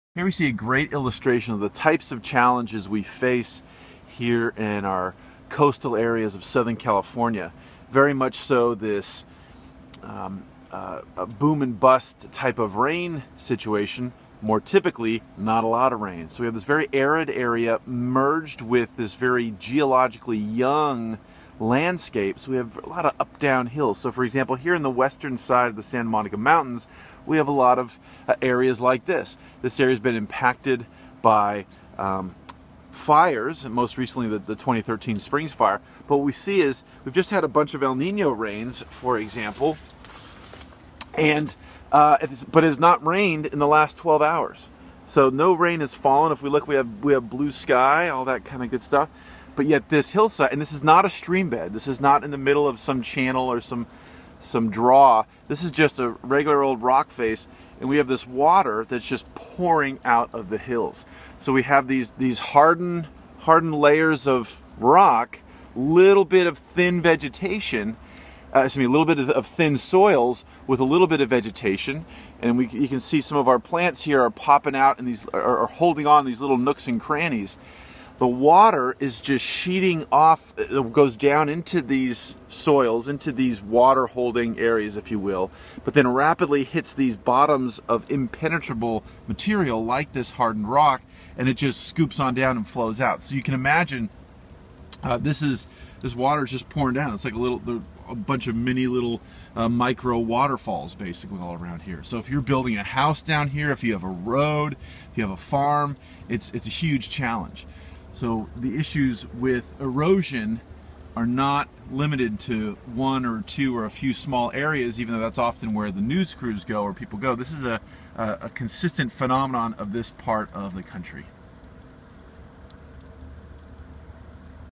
Water-Shedding Hills
Here I illustrate the poor water retention in some of our coastal hillsides.  This was recorded more than 12 hours after the last rains on the western edge of the Santa Monica Mountains.